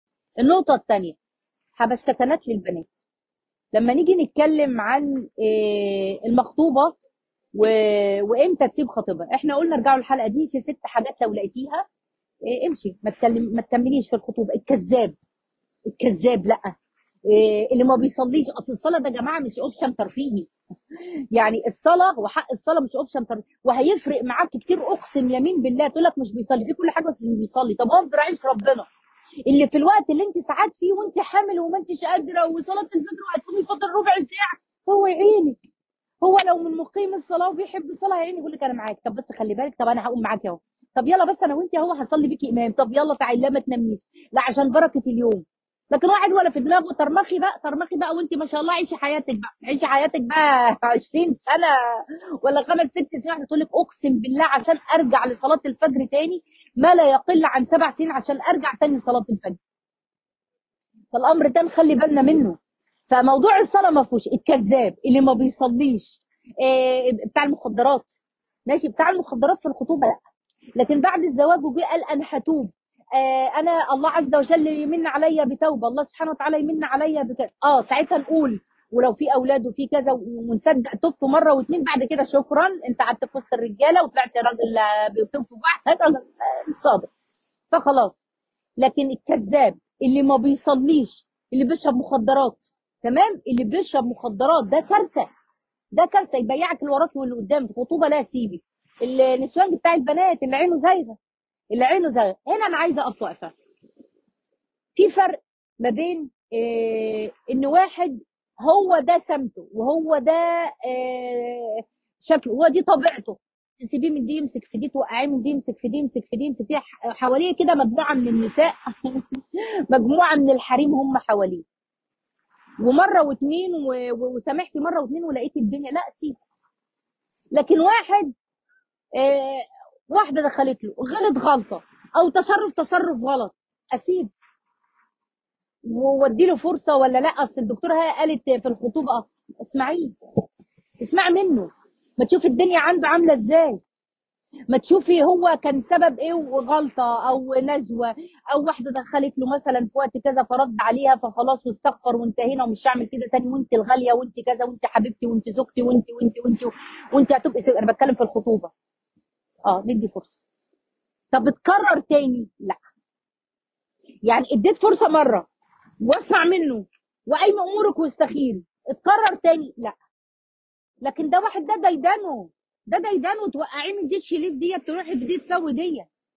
إمتي تسيبي خطيبك ومتكمليش، 6 عيوب لو لاقتيهم فيه متكمليش ... كلام مهم للبنات مع د. هالة سمير
shorturl.at/afwD9